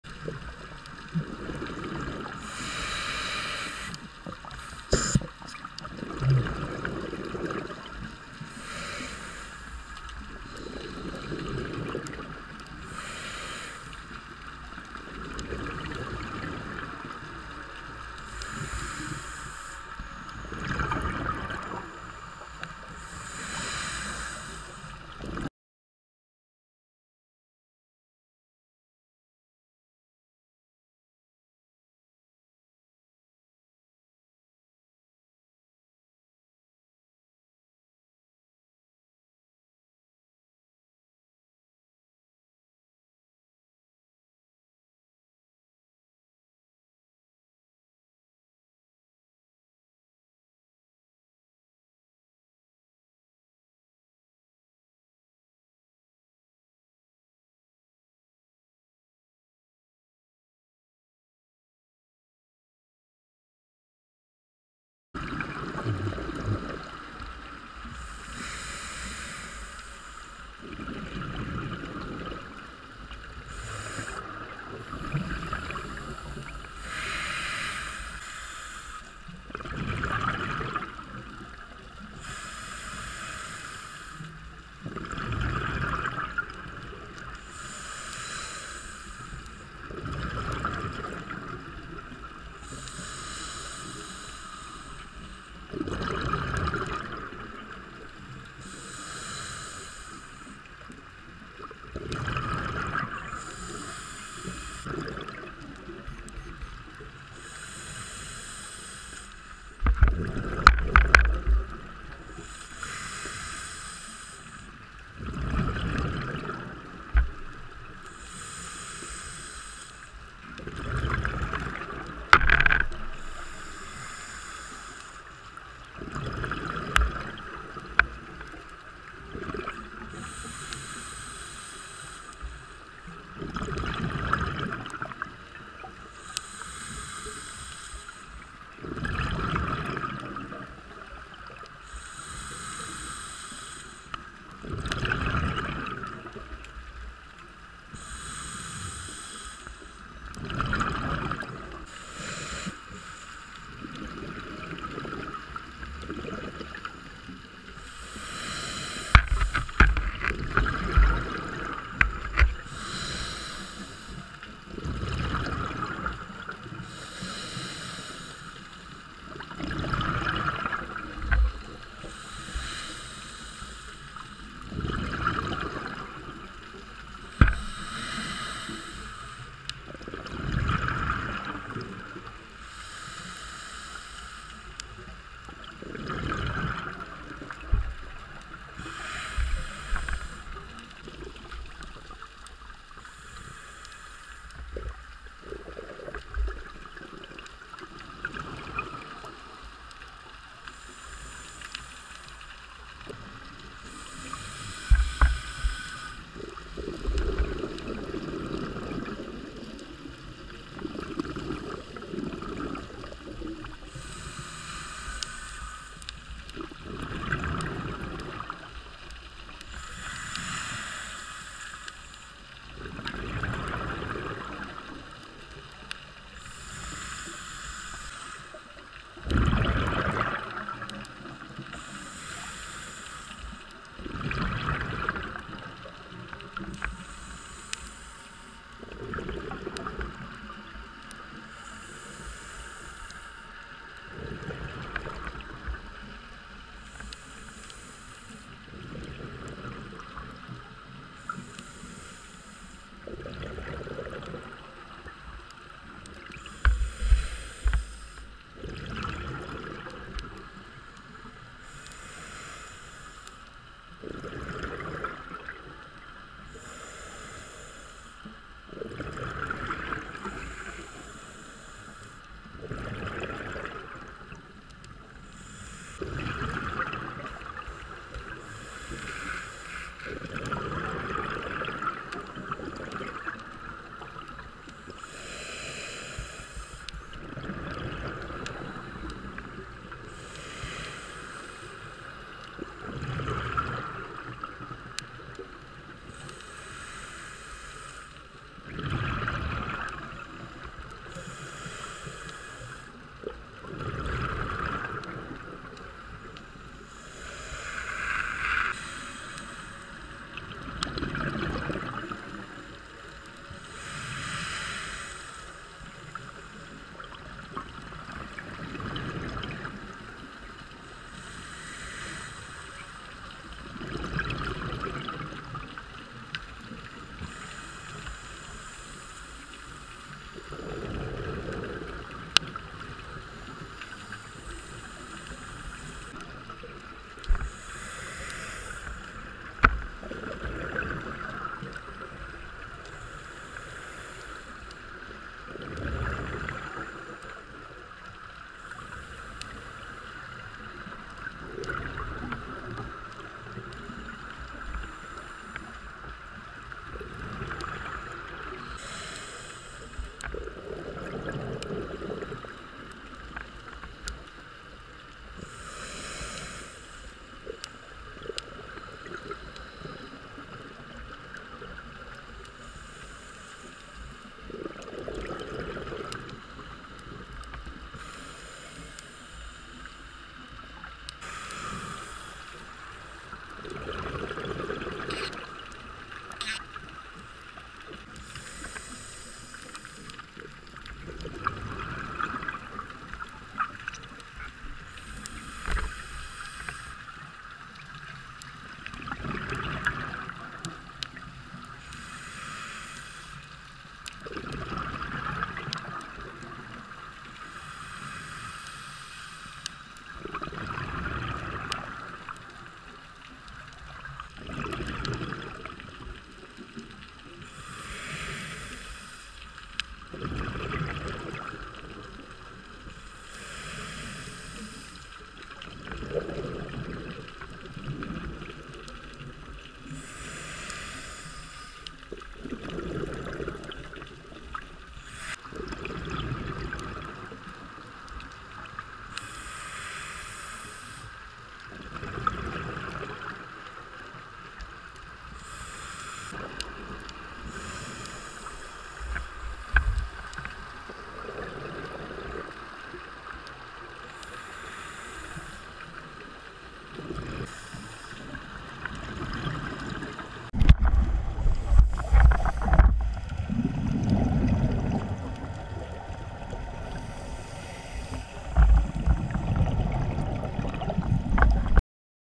Wreck dive, Fujairah
2nd Wreck dive around Fujairah with Alboom diving center On Jan. 2nd 2014.